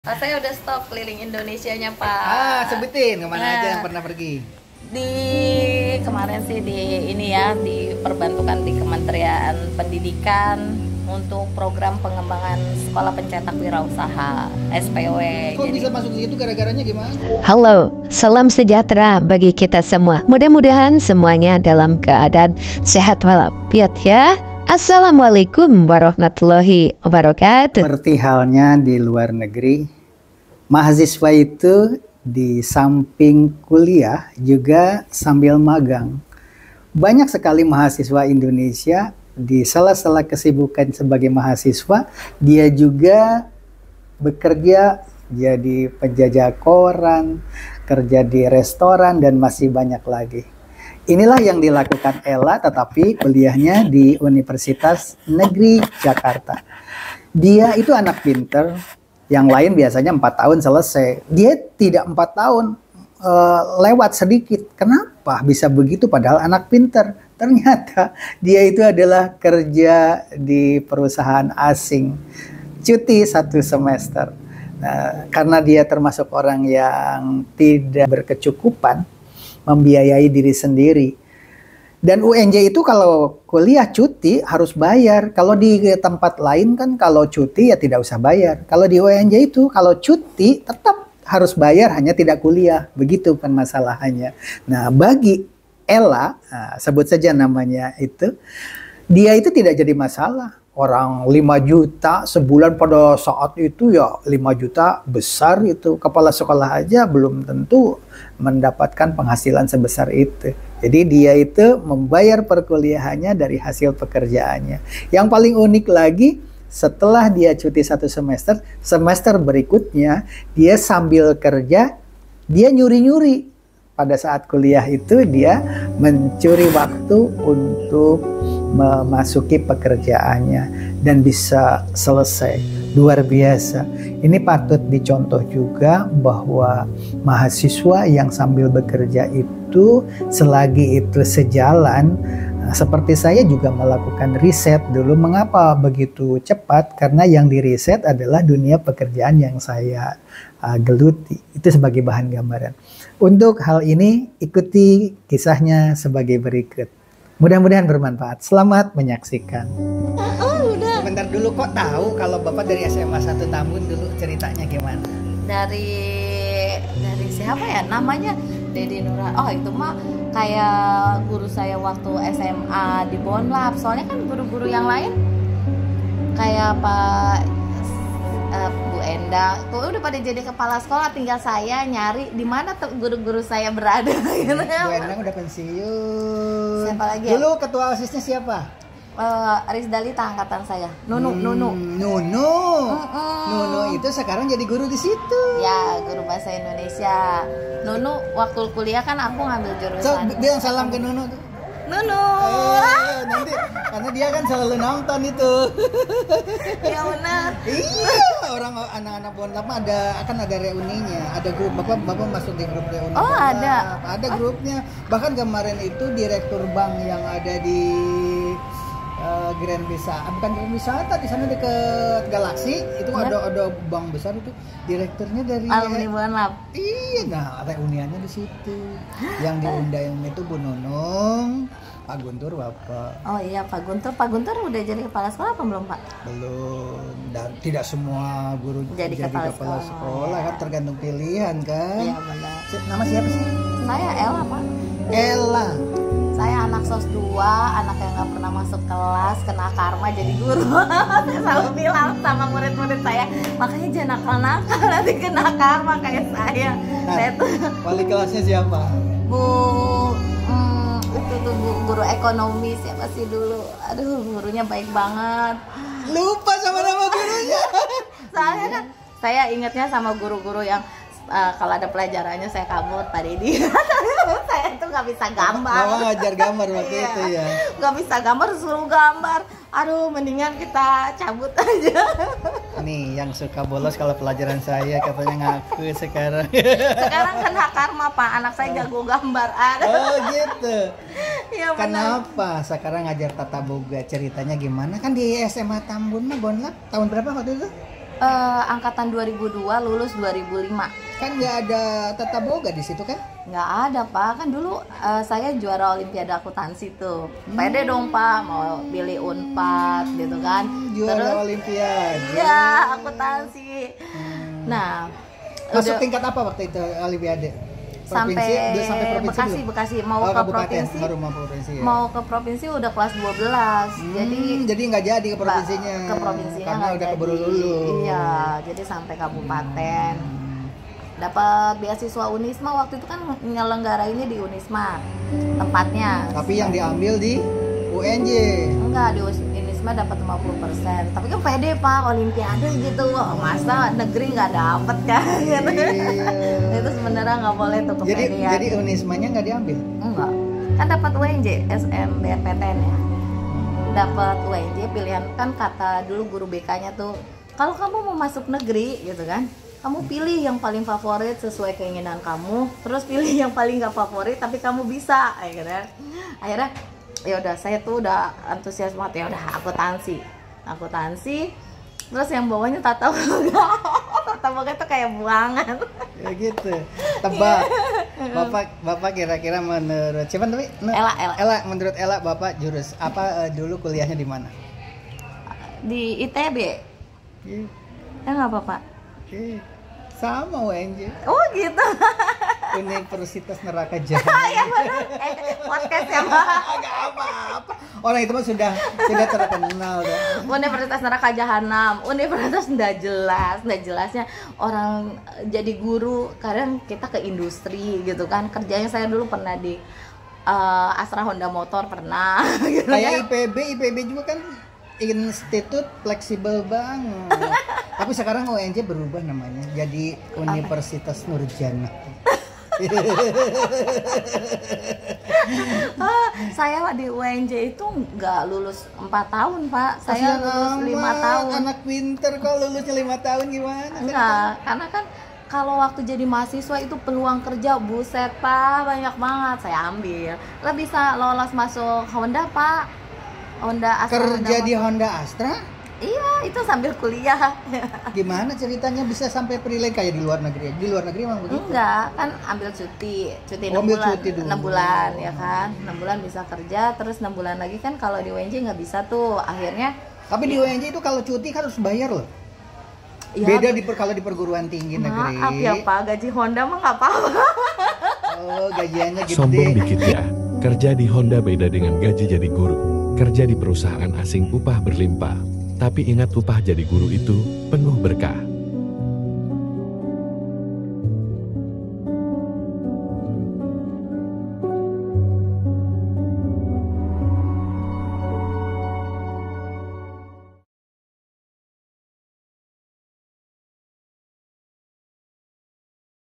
Ella: Keliling Indonesia."Tata Boga" Pilihan Akhir. Kuliah Sering Bolos, Mengapa?
Ella: Keliling Indonesia,.Tata Boga Pilihan Akhir. Kuliah Sambil Kerja, Mengapa?